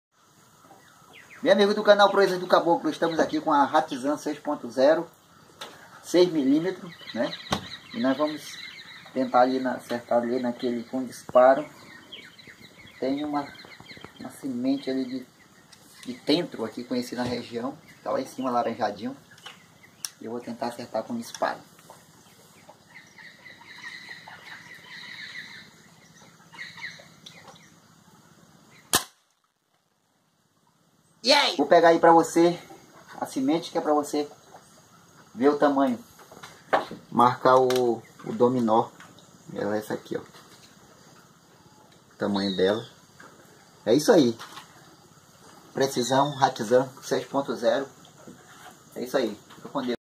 Hatsan 6.0 acertando semente de tenturana
Vídeo de Benaia Cruz